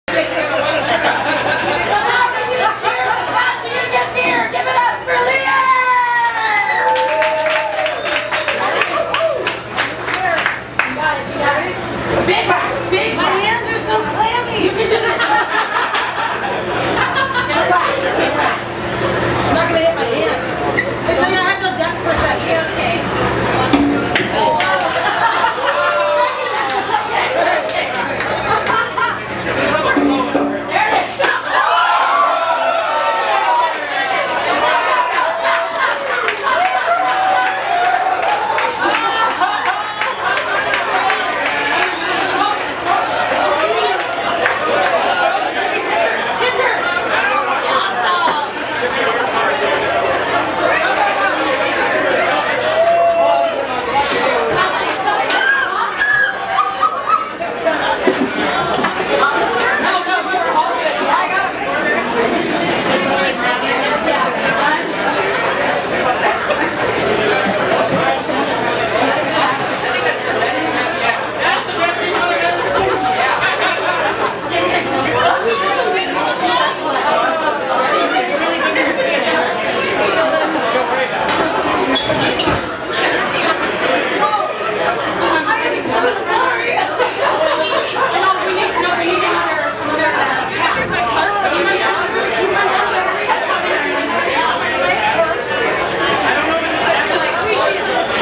The Best Worst Firkin Tap
I'm make no claim to be a beer buff, nor have I ever tapped a firkin. This is the opening of my First Friday creative photography show. I'm sure this must be the most epic, awful, blow out on a firkin keg... ever. Turned The Loft into a temporary Sockeye Red splash zone. In all fairness, the beer was really, really carbonated and the bung wasn't seated quite right... So the brewers told me... after the fact. Yeah. Of all the epic keg malfunctions that could have happened, I'm glad it happened to me, right here, with such great company. Most memorable First Firkin Friday ever.